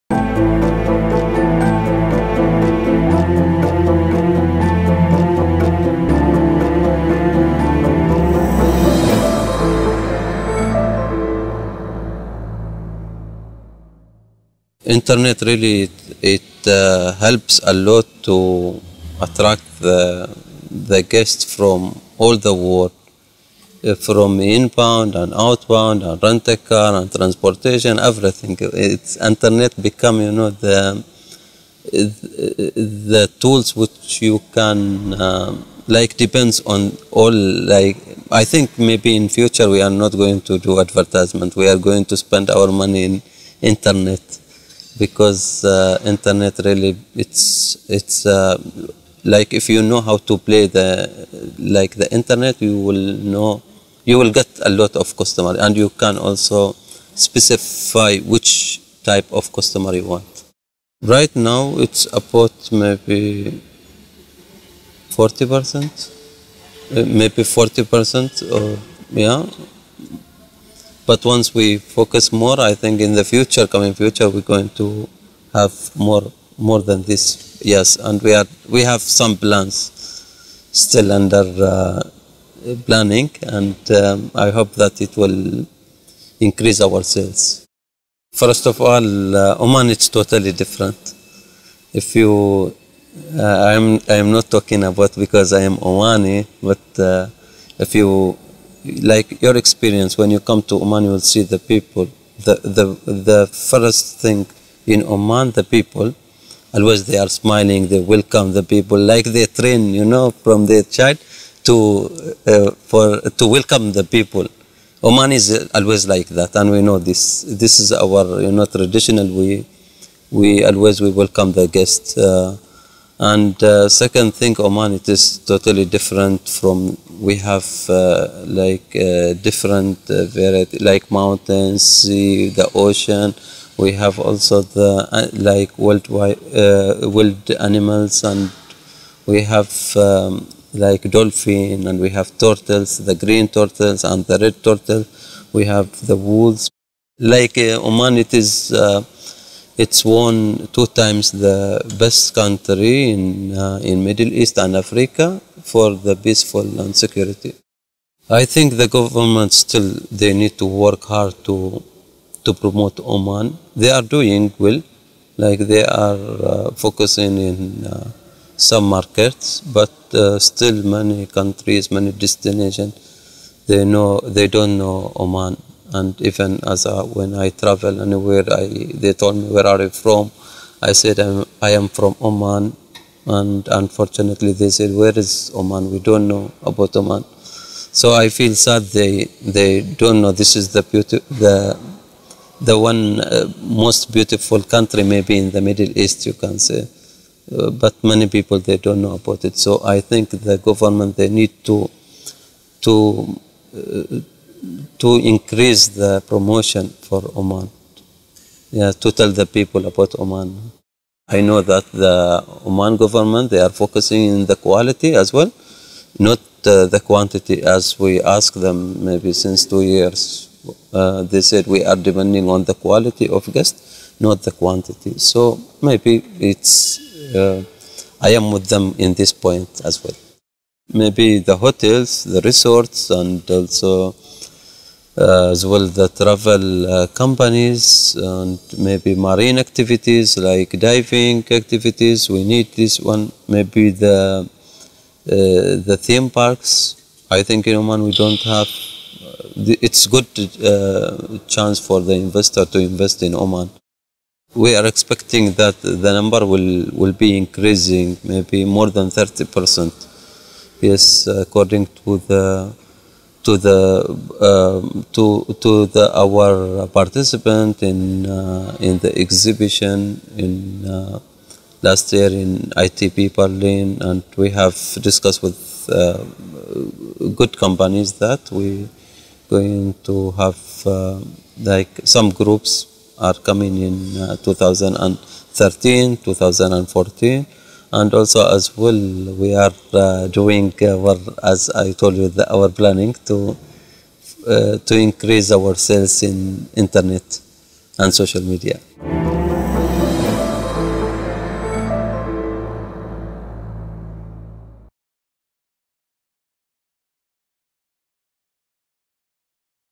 Alwan Travel & Tourism General Manager Talal Al Rawahi on Oman's tourism industry
Talal Al Rawahi is the General Manager of Alwan Travel & Tourism, a tourist agency in the Sultanate of Oman. He spoke with The Prospect Group about the importance of the Internet in the tourism industry, Oman's competitive advantages vis-à-vis other countries in the region, and the investment opportunities available in the tourism sector.